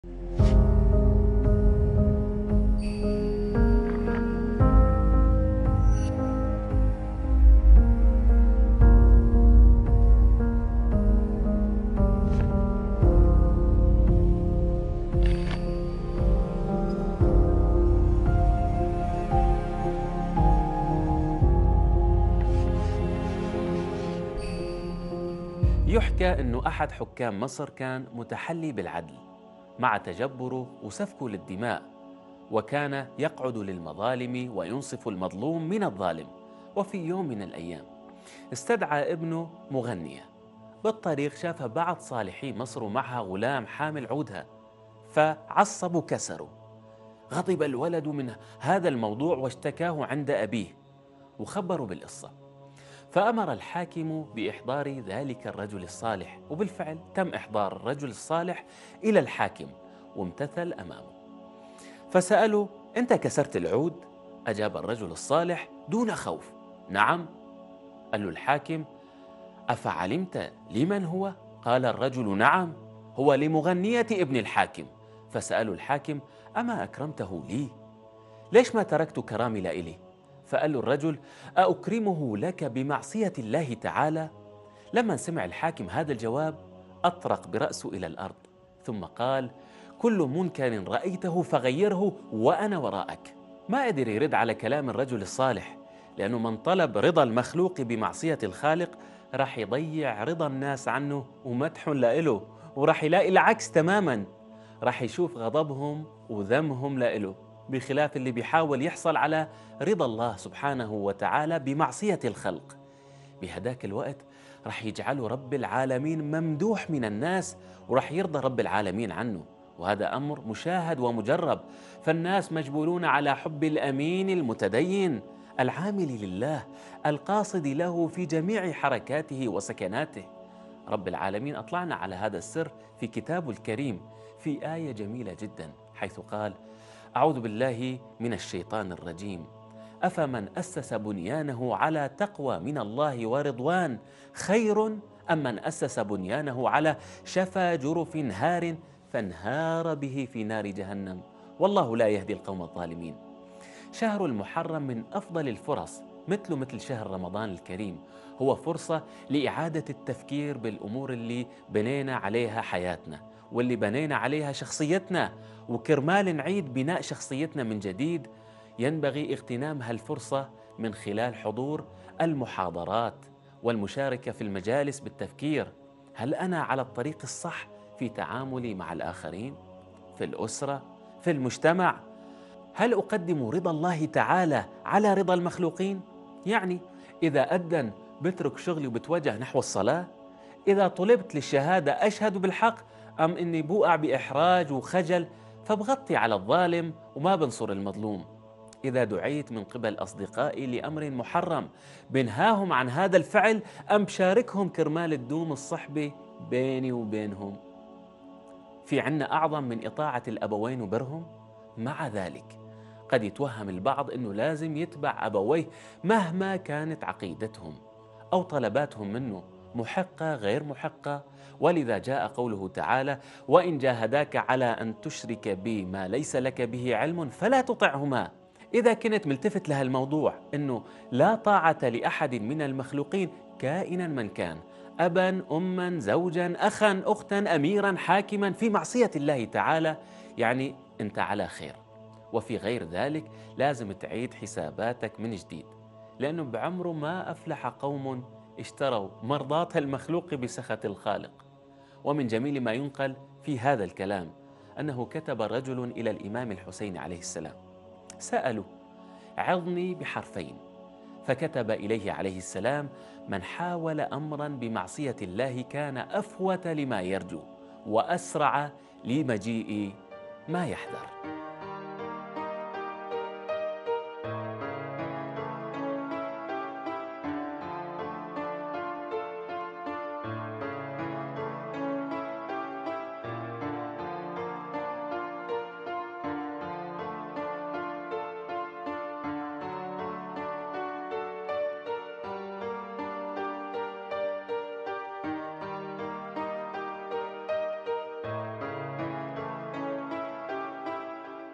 كلمتين وبس - قصة الرجل الصالح الذي نهى بن الحاكم عن المنكر
5 المحرم الحرام 1445 هـ